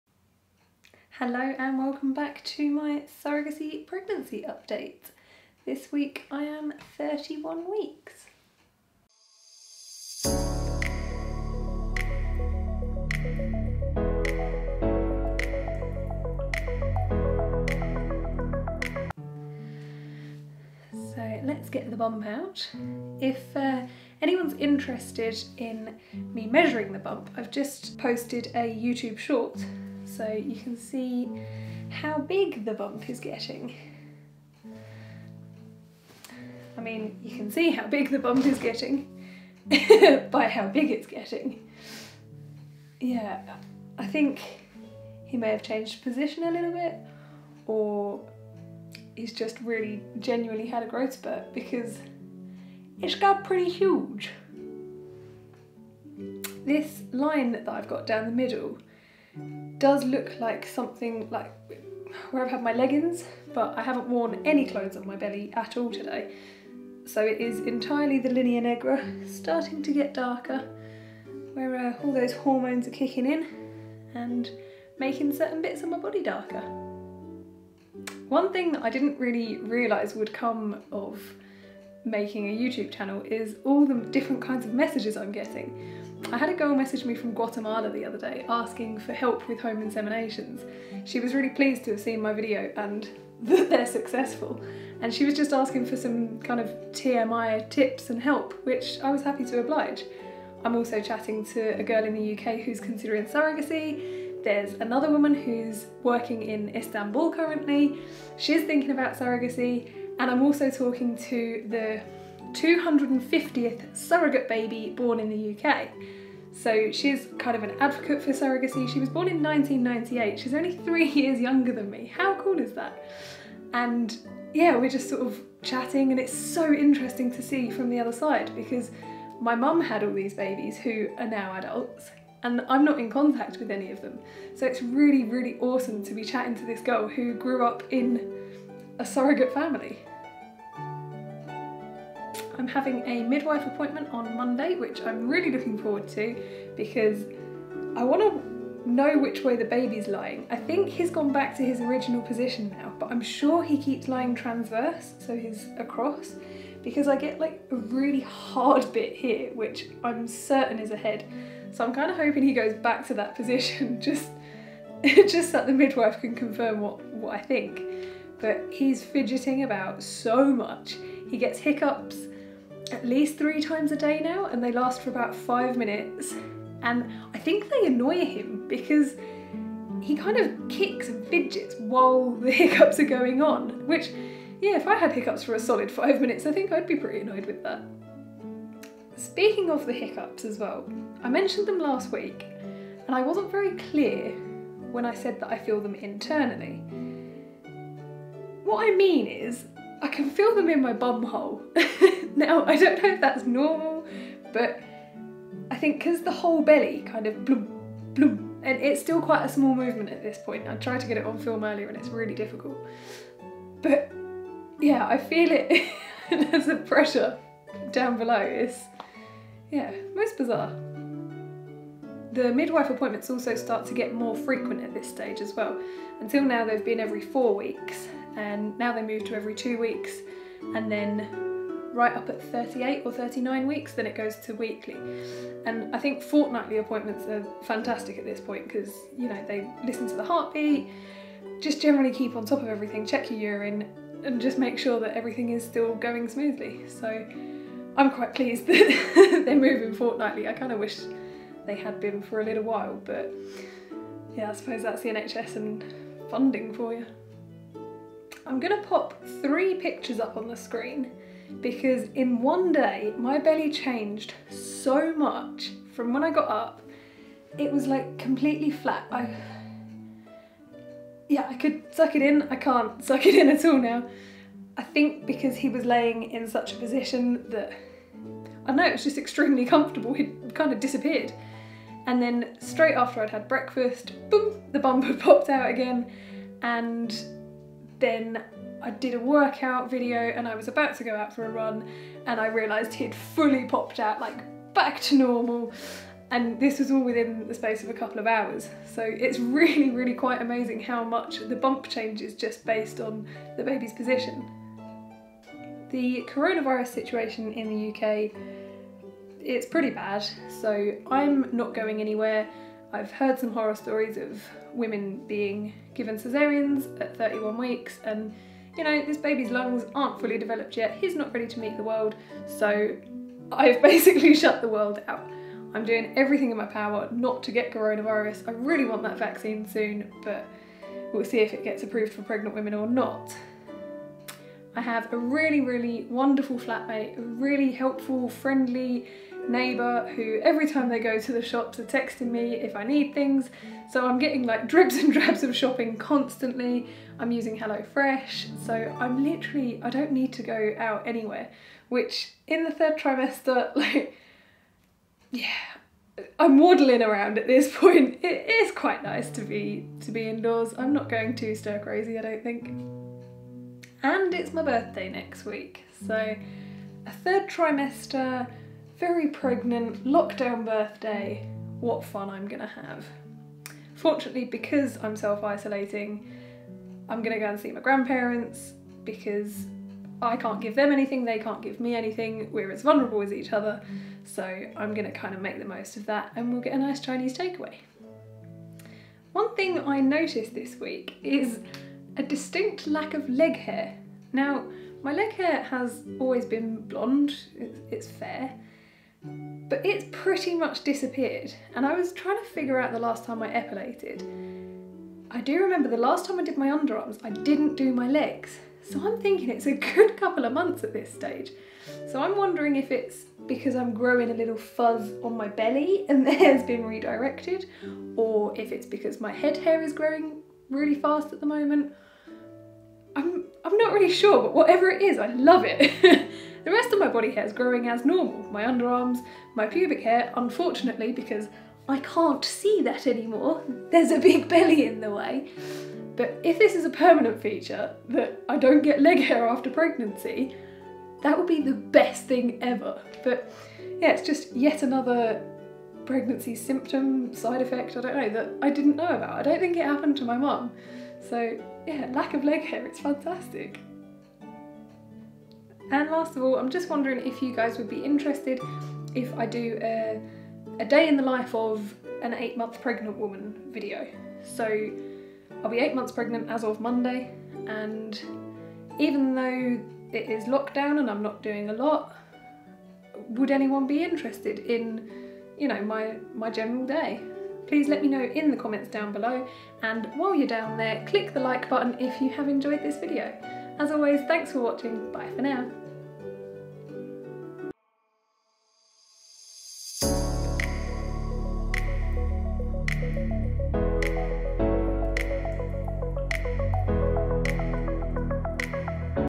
Surrogacy bump update | 31 weeks pregnant
This week I talk about my linear nigra getting darker as well as the baby's hiccups and movements... Including a possible TMI of where I feel these movements from the inside.

I have had the pleasure of chatting to some amazing women around the world thanks to this YouTube channel, and hopefully I am encouraging some women to consider becoming surrogates!

Midwife appointments are becoming more frequent and I am looking forward to the reassurance - as I'm sure the baby's parents are too. They will be on video call!

One day this week my belly changed so much in just a couple of hours. The baby had practically disappeared when I woke up and then fully popped out again after some breakfast and exercise!

Lastly, body hair! My leg hair has all but disappeared and my underarm and pubic hair has definitely slowed in growth. There are so many other factors to pregnancy I had never even considered!

As always - I strip off to my undies to show you lot how I'm getting along in my first ever pregnancy.

About me:
I'm British and a traditional surrogate (TS) which means I used my own egg to create a family for a couple. Extreme babysitting for 9 months, followed by a year's maternity leave when I will travel the world 🌍